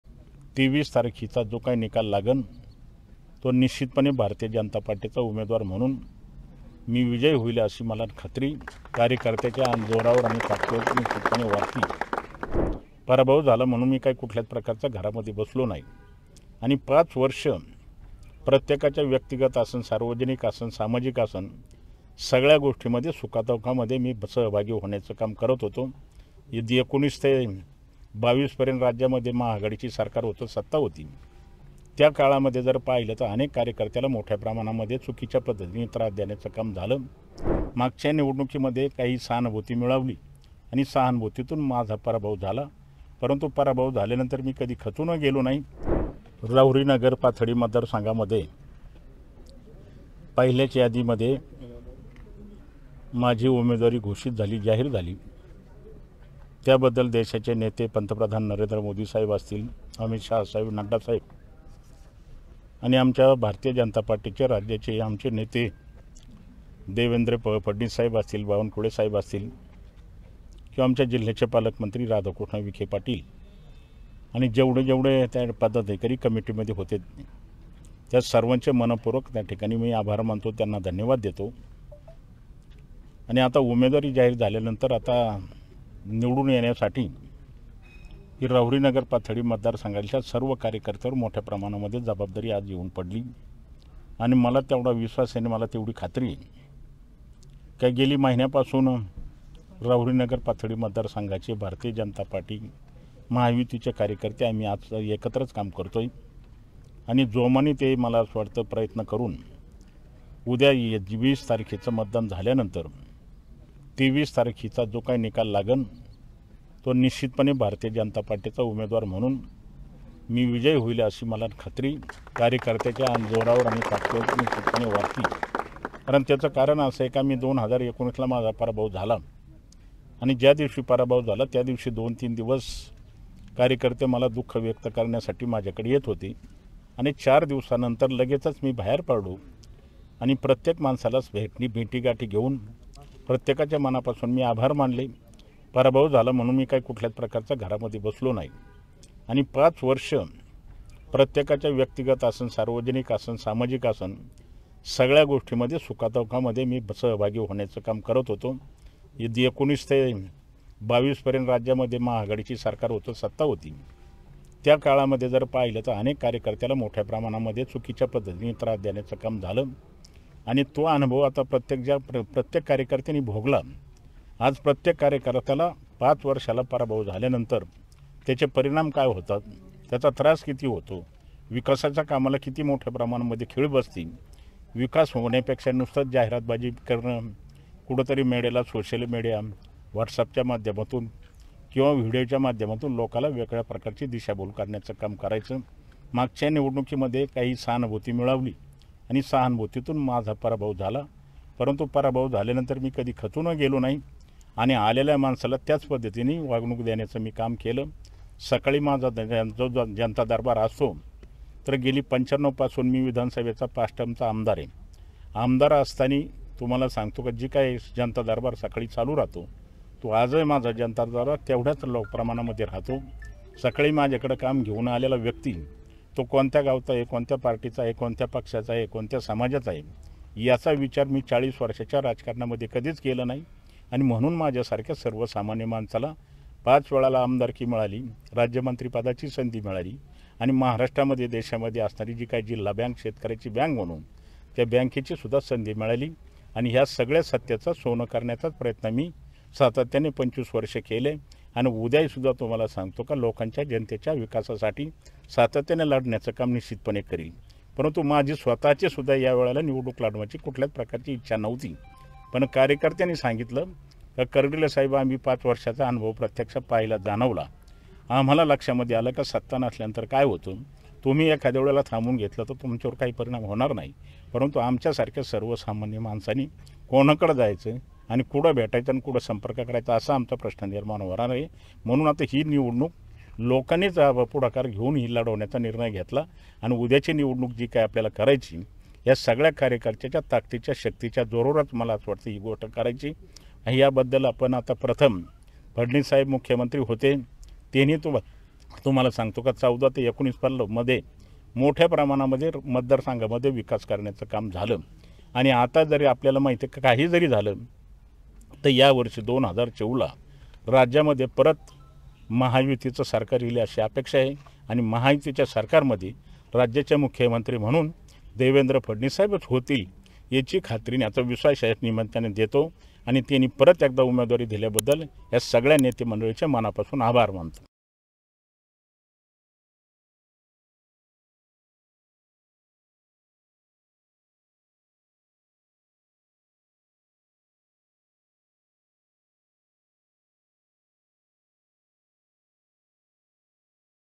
Shivaji Kardile | महायुतीचंच राज्य अन् फडणवीसचं मुख्यमंत्री; कर्डिलेंना विश्वास | LetsUpp Marathi
राज्यात पुन्हा महायुतीचं सरकार स्थापन होणार असून देवेंद्र फडणवीस मुख्यमंत्री होणार असल्याचं म्हणत उमेदवारी मिळाल्याबद्दल भाजपच्या वरिष्ठ नेत्यांचे शिवाजीराव कर्डिलेंनी आभार मानले आहेत.